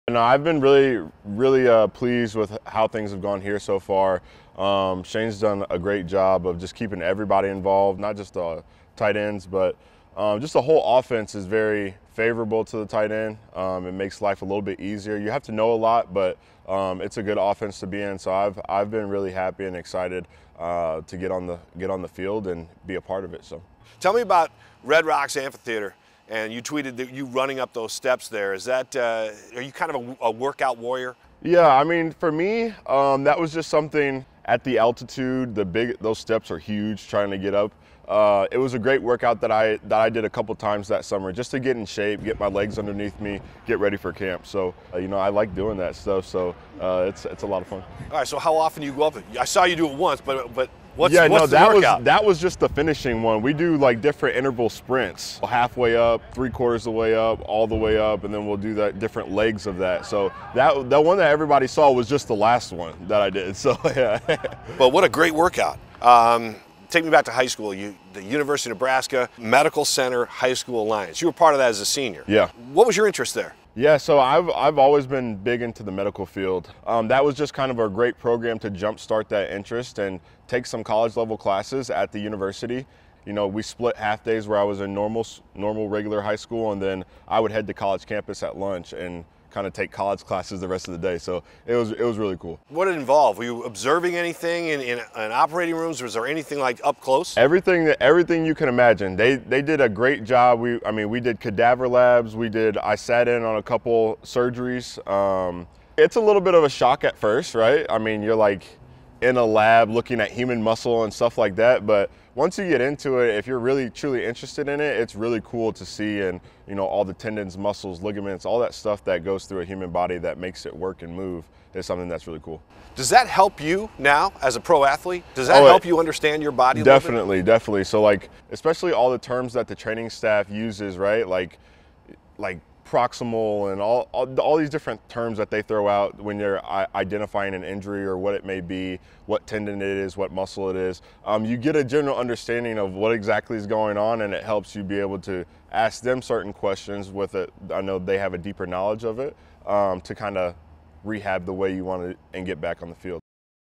TE Noah Fant on Seahawks offense, running at Red Rocks
Seahawks tight end Noah Fant sits down with Paul Silvi to talk about this season’s offense, his workout routine and his interest in the medical field.